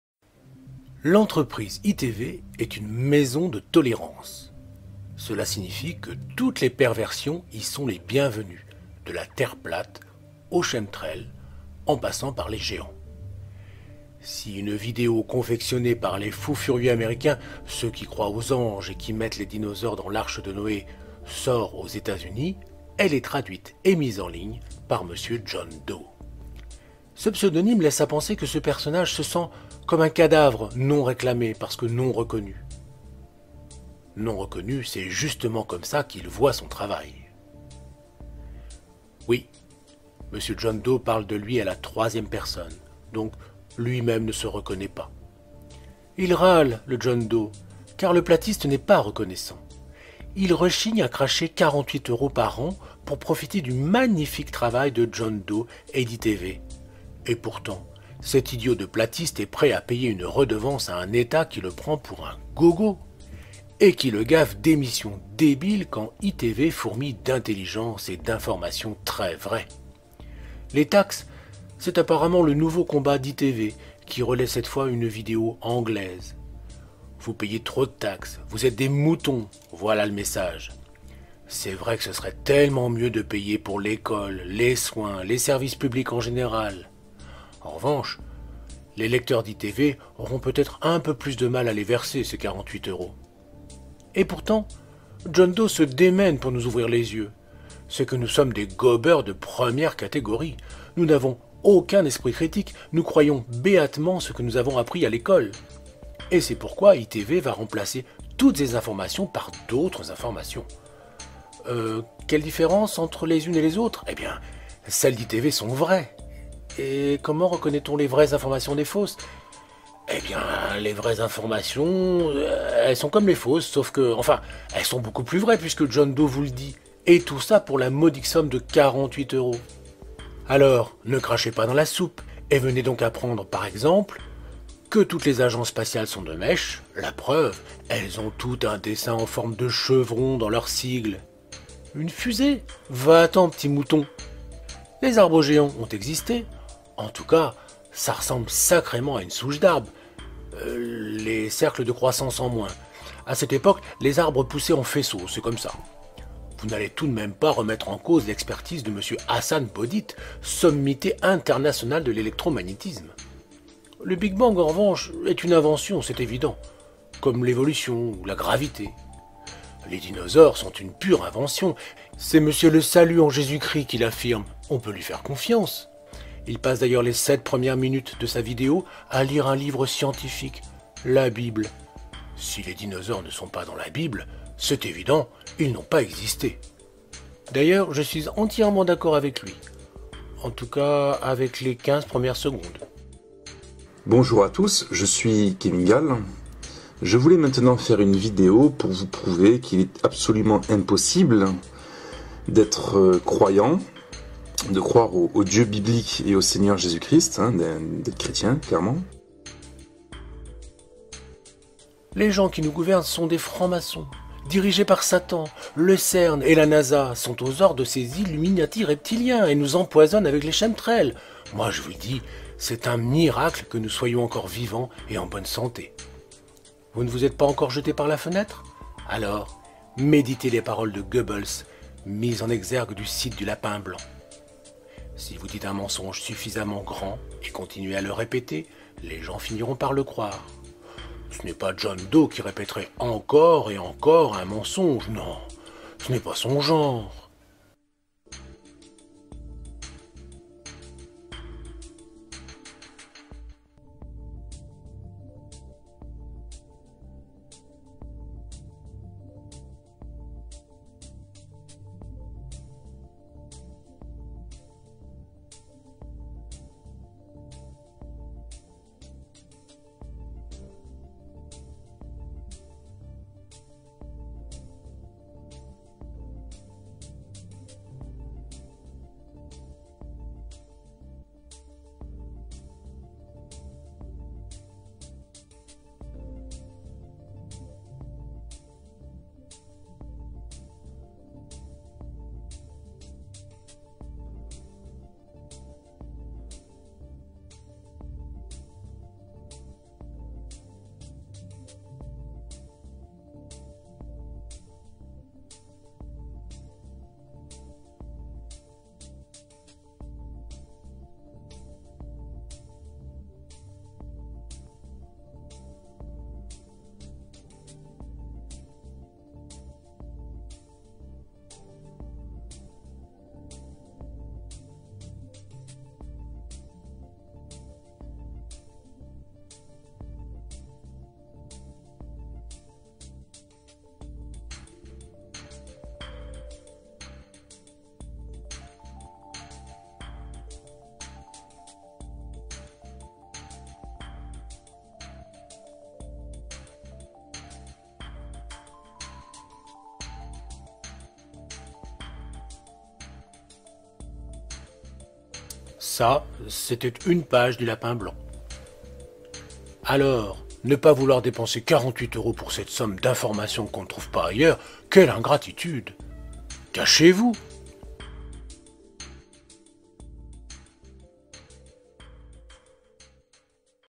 « L'entreprise ITV est une maison de tolérance » – Quand ITV nous informe
Une fois n'est pas coutume, je reprends une vidéo d'une autre chaîne, celle de François, avec son autorisation.

François fait du bon travail sur les conspinouilles de la Terre plate et je vous invite à regarder et à liker ses vidéos

Son analyse d'ITV est très fine et percutante.

Et nous sommes d'accord sur un point essentiel : ITV est un proxénète.